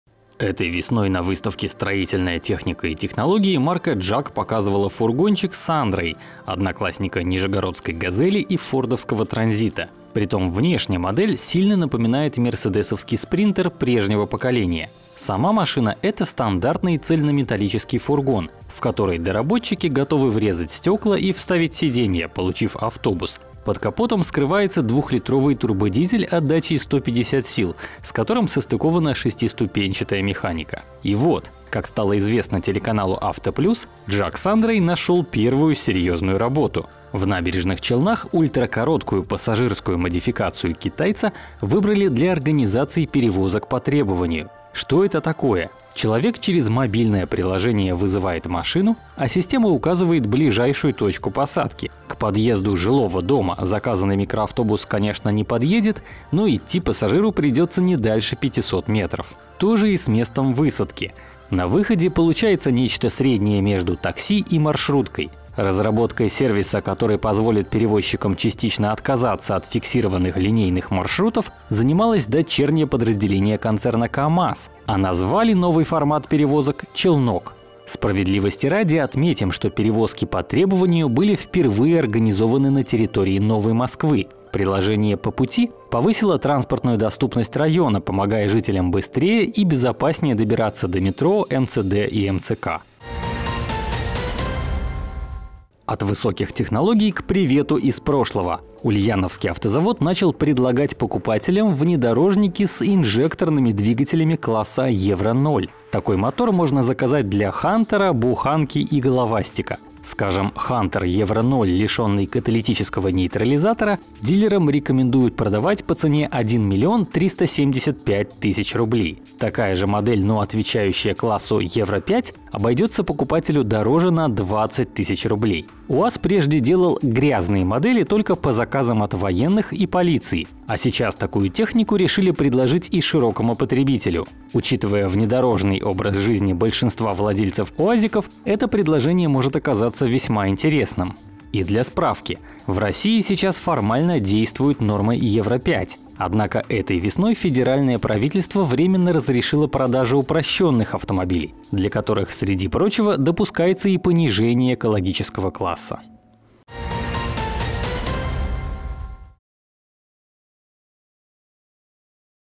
JAC Sunray нашёл серьёзную работу. УАЗ Хантер предложит «грязную» версию | Новости с колёс №2170
Этой весной на выставке «Строительная техника и технологии» марка JAC показывала фургончик Sunray — одноклассника нижегородской ГАЗели и фордовского Транзита. Притом внешне модель сильно напоминает мерседесовский Спринтер прежнего поколения. Сама машина — это стандартный цельнометаллический фургон, в который доработчики готовы врезать стёкла и вставить сиденья, получив автобус. Под капотом скрывается двухлитровый турбодизель отдачей 150 сил, с которым состыкована шестиступенчатая «механика». И вот, как стало известно телеканалу «Авто Плюс», JAC Sunray нашёл первую серьёзную работу. В Набережных Челнах ультракороткую пассажирскую модификацию «китайца» выбрали для организации «перевозок по требованию». Что это такое. Человек через мобильное приложение вызывает машину, а система указывает ближайшую точку посадки. К подъезду жилого дома заказанный микроавтобус, конечно, не подъедет, но идти пассажиру придётся не дальше пятисот метров. То же и с местом высадки. Система выберет оптимальную геопозицию, чтобы клиенту было недалеко шагать до места назначения. На выходе получается нечто среднее между такси и маршруткой. Цена такой услуги тоже будет «половинчатой». Разработкой сервиса, который позволит перевозчикам частично отказаться от фиксированных линейных маршрутов, занималось дочернее подразделение концерна КамАЗ. А назвали новый формат перевозок «Челнок». Справедливости ради отметим, что перевозки по требованию были впервые организованы на территории Новой Москвы. Приложение «По пути» повысило транспортную доступность района, помогая жителям быстрее и безопаснее добираться до метро, МЦД и МЦК. Автобус можно вызвать через приложение «Московский транспорт», и в течение 30 минут он подъедет на «виртуальную» остановку, расположенную не дальше пятисот метров от места заказа. Сейчас система начинает работать и в исследовательском центре «Сколково».

От высоких технологий — к привету из прошлого. Ульяновский автозавод начал предлагать покупателям внедорожники… с инжекторными двигателями класса Евро-0. Такой мотор можно заказать для Хантера, «буханки» и «головастика». Скажем, Хантер Евро-0, лишённый каталитического нейтрализатора, дилерам рекомендуют продавать по цене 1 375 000 рублей. Такая же модель, но отвечающая классу Евро-5, обойдётся покупателю дороже на 20 000 рублей. УАЗ прежде делал «грязные» модели только по заказам от военных и полиции. А сейчас такую технику решили предложить и широкому потребителю. Учитывая внедорожный образ жизни большинства владельцев «уазиков», это предложение может оказаться весьма интересным. И для справки. В России сейчас формально действуют нормы Евро-5. Однако этой весной федеральное правительство временно разрешило продажи упрощённых автомобилей, для которых среди прочего допускается и понижение экологического класса.

00:00 JAC Sunray нашёл первую серьёзную работу
01:35 УАЗ Хантер предложит «грязную» версию